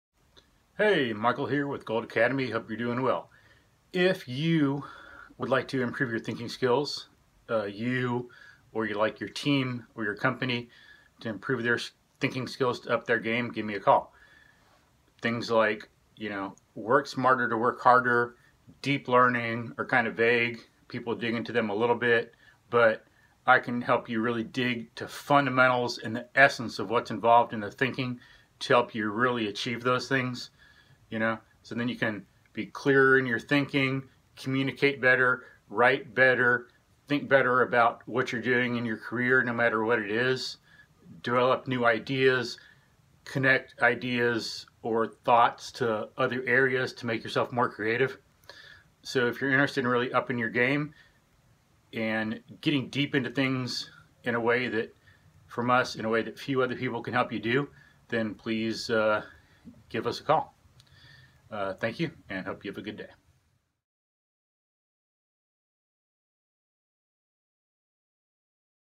Gold Academy Gives You Thinking Skills for Work and Life 1-16–2018
"Prior to working with Michael, I had very little hope of actually pursuing my dream to work in the aerospace industry as an engineer.  Yet here I am, finishing an engineering degree at A&M this Fall and working at Bell Helicopter as an engineer.

"I wasn't good at math.  I didn't like math (in fact, I avoided it). With Michael's help, I came to understand the importance of not only mathematics, but also reason, rationality, and a constant pursuit of knowledge.  His emphasis on critical thinking and questioning changes the fundamental way student's think about and approach problems, regardless of the specific nature of the problem.

"In other words, don't expect him to do your homework -- expect to come out asking for seconds. You will take away a lot more than you ever expected, and it will stick with you for a long time.

"I honestly wish more teachers were like Michael -- maybe they should take a lesson or two!

"I cannot recommend Michael enough for any student of any subject; this guy is simply the best." — Drew T

Find out more on:
1.  Facebook
2.  Yelp

Michael can help you get that better grade — but is one of the rare teachers who can give you an understanding of a concept or subject.

What’s more, he’s one of the even rarer teachers who can teach you and train you in reason and logic, which are what you really need to do well in school and out.

Because what matters ultimately is what you do at work and the life you live.

And we ain’t talking that nonsense pseudo “reason” or “logic” used only in school books, cocktail parties, university classrooms, or Spock’s head. We are talking the stuff that matters in the real world and out in the street. Stuff that is practical, useful, powerful, effective, and vital.